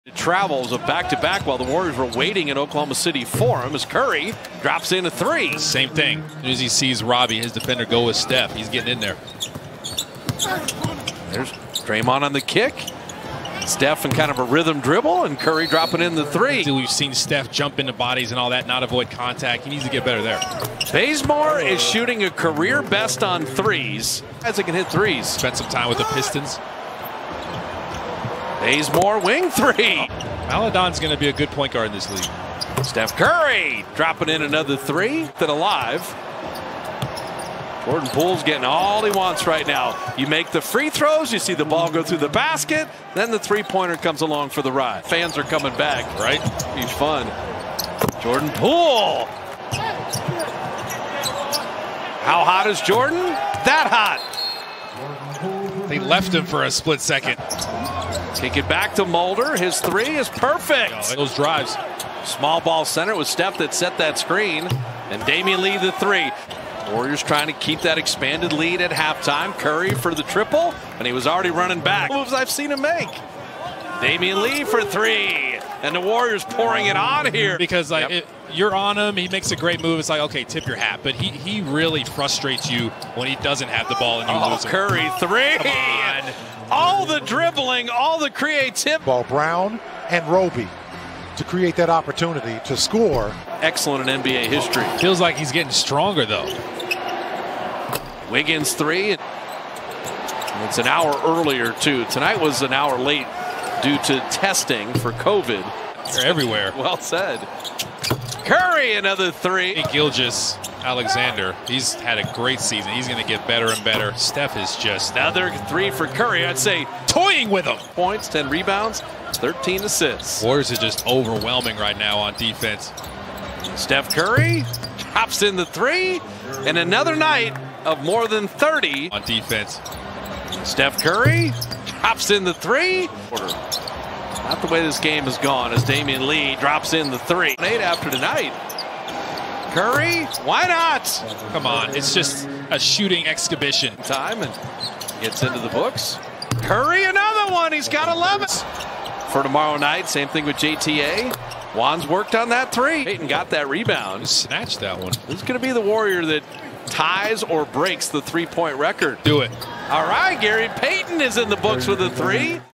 SPLASH PARTY! Warriors Tie Franchise Record with 24 Threes | April 14, 2021
Welcome to our SPLASH party.

The Warriors equaled a franchise record with 24 made threes tonight in Oklahoma City ☔️